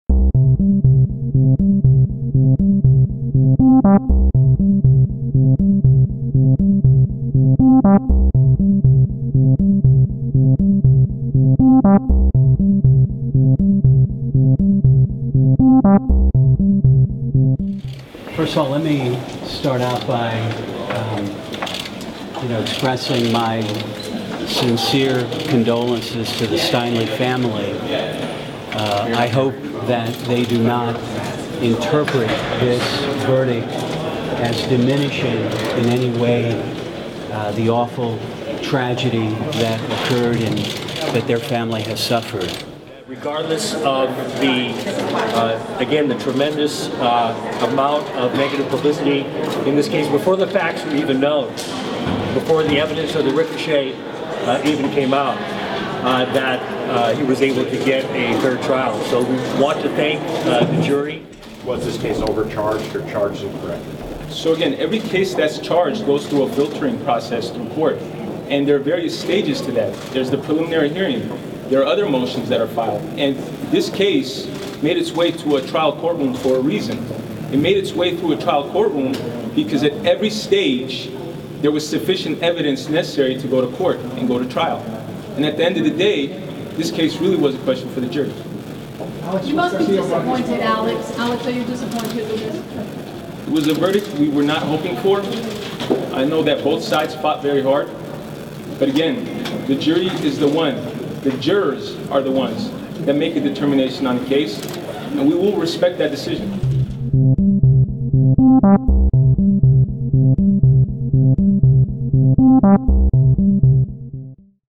Attorneys weigh in on verdict in Kate Steinle case
Public defenders Matt Gonzalez and Jeff Adachi say Jose Ines Garcia Zarate received a fair trial in San Francisco. Alex Bastian of the District Attorney's Office said he was disappointed with the case.